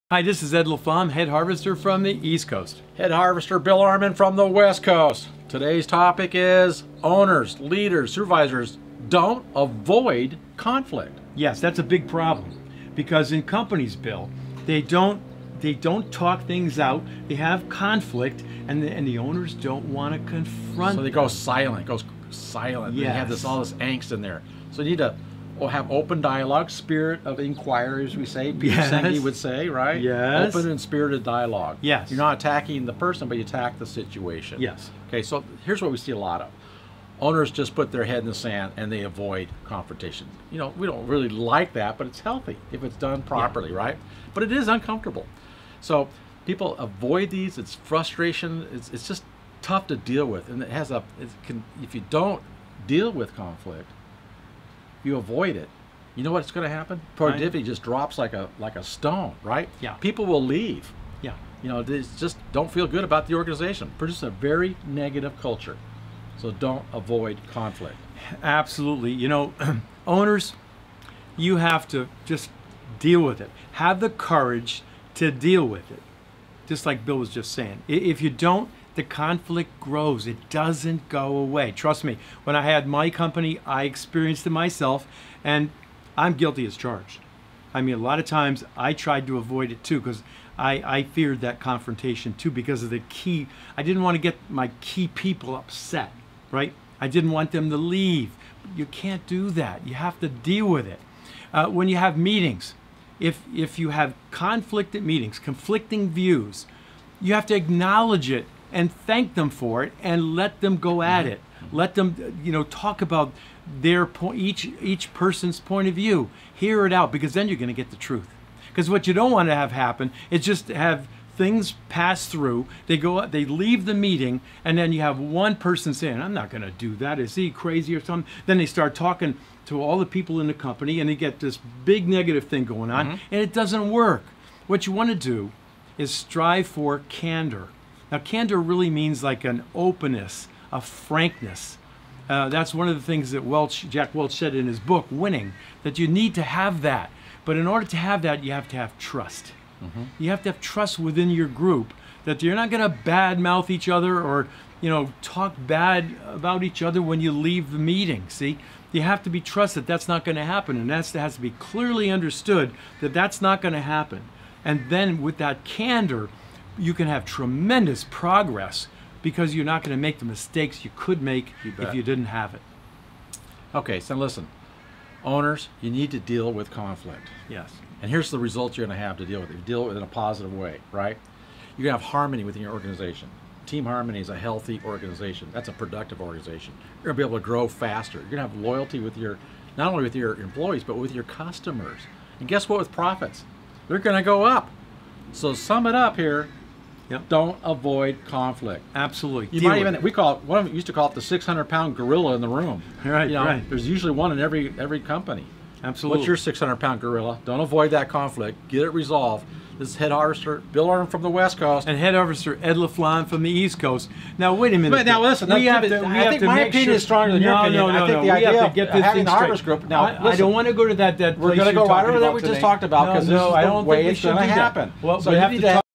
Owners & Managers — Don’t Avoid Conflict
The Harvesters are going to talk about a “sticky”subject - conflict.  Do you have conflict in your organization? Of course you do, every company does, the question is how do you deal with it? Do you allow conflict at your meetings? Should you? How do you deal with it?

Watch their quick video and they will help with some solid advice. The Harvesters hope these videos help you, Harvest YOUR Potential!